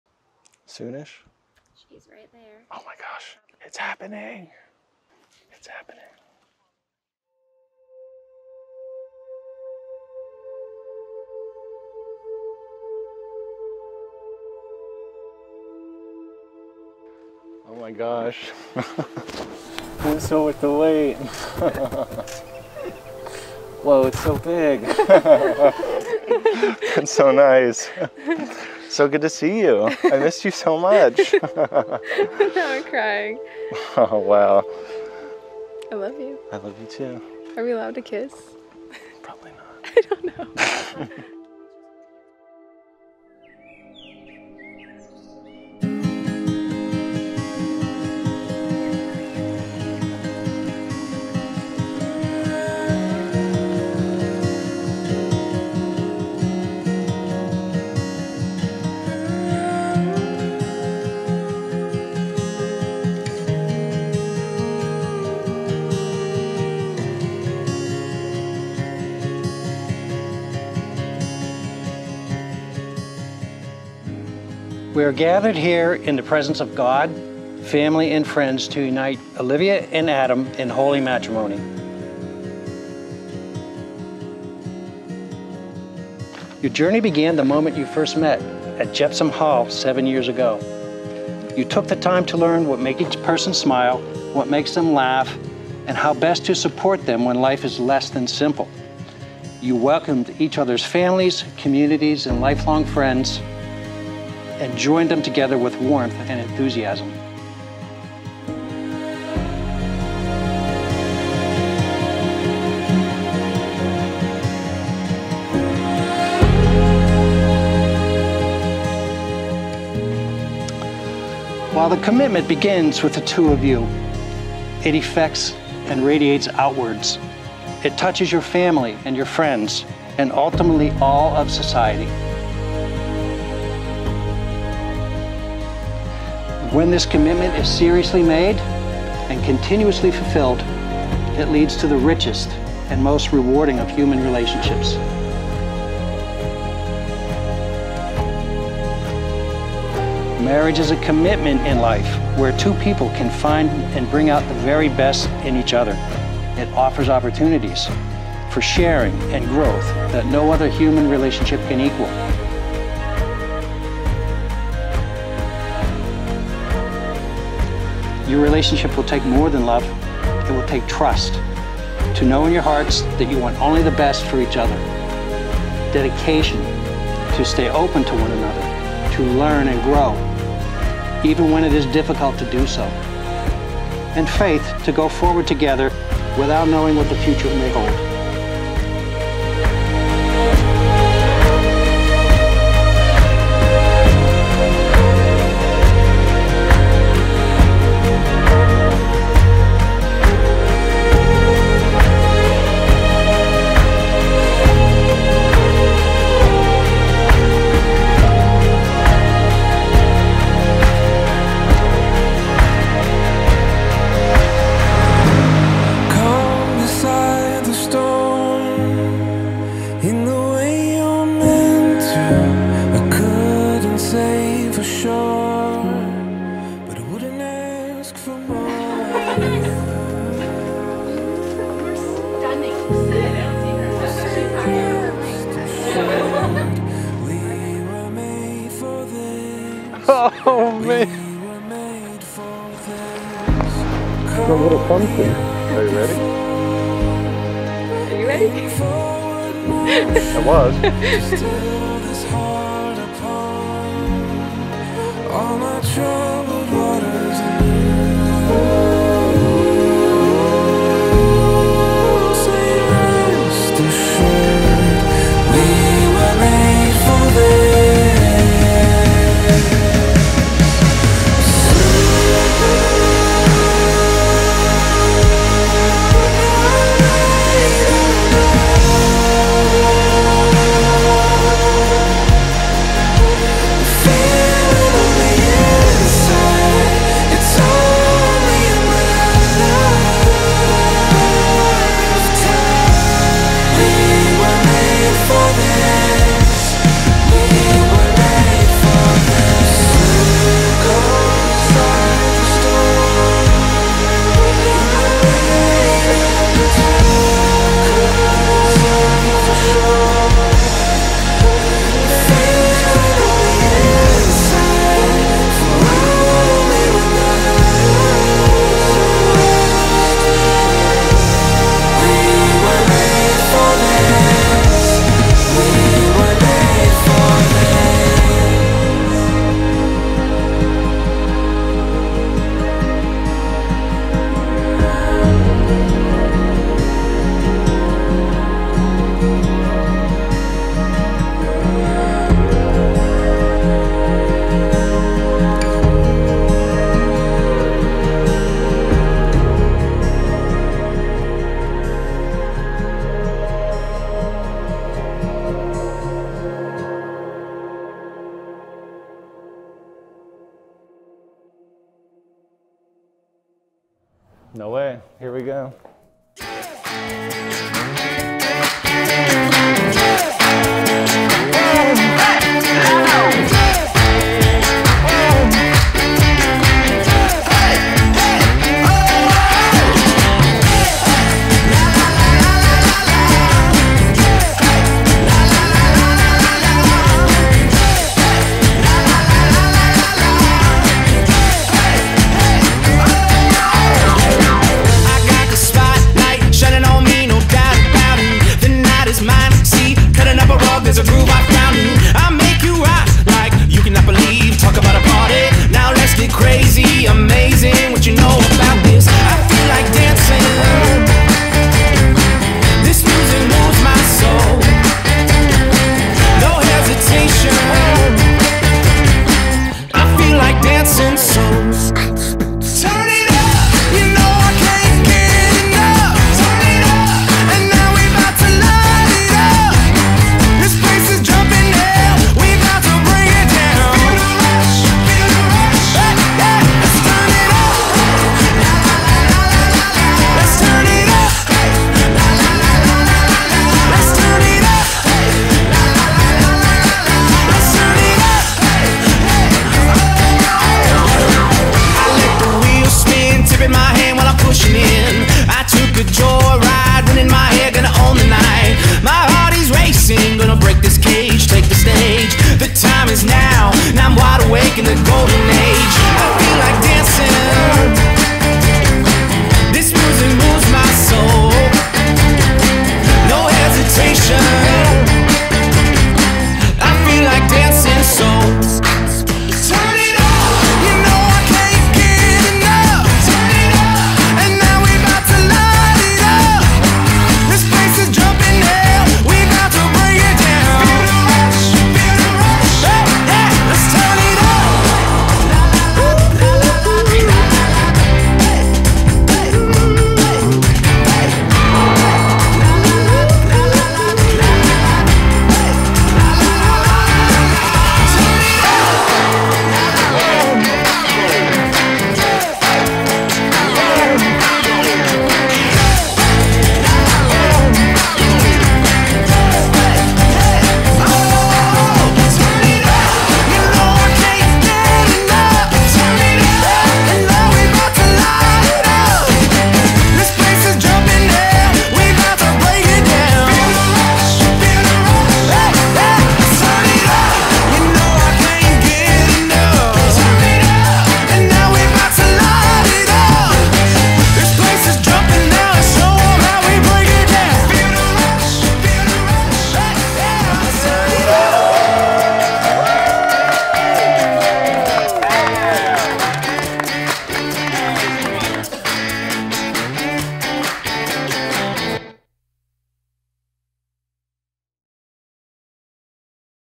Olivia + Adam
These two met in Chemistry class in college and I think it’s the most fitting beginning to their story. Not only was their love for each other very apparent, but their friendship was so much fun to capture.
Thank you so much Olivia and Adam for having me be a part of this amazing day. It’s one I will never forget!

Venue: The Lodge at Liberty Forge
Photography: Amber Urey Photography
Videography: Maria Miller Media
Dress: Ava Laurenne Bride
Cake: The Pennsylvania Bakery
DJ: Klock Entertainment
Florist: Blooms by Vickrey